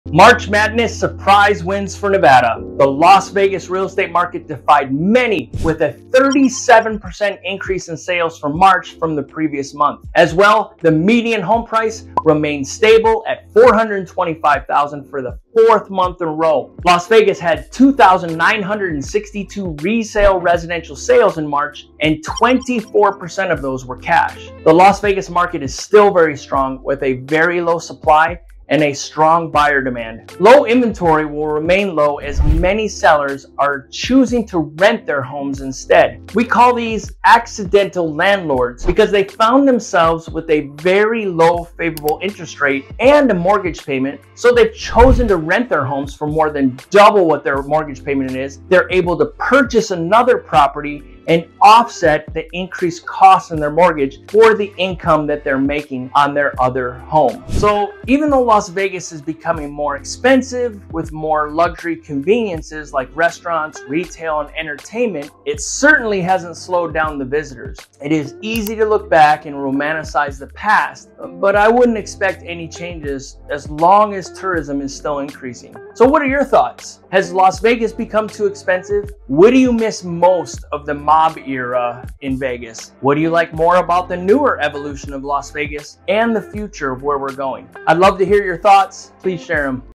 March Madness Las Vegas Market Update 2023
Las Vegas proved everyone wrong and put up some Large numbers in March 2023 with a 37% increase in home sales from February. Median home price remained stable at $425,000 for the 4 month in a row. Inventory is low and Buyer demand still strong. Will prices start to increase as interest rates trickle downward?